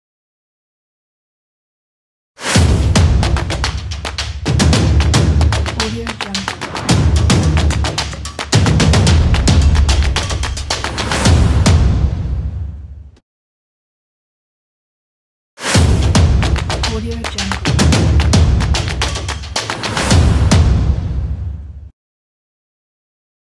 Rhythmic Intro | After Effects Template | Project Files - Videohive
Features

2 versions: 12 seconds, 8 seconds
Fast render
Color control
AE CS5, AE CS5.5, AE CS6, AE CC
Contains video tutorial
Images and music not included

FEATURES
Length 0:12
Resolution 1920x1080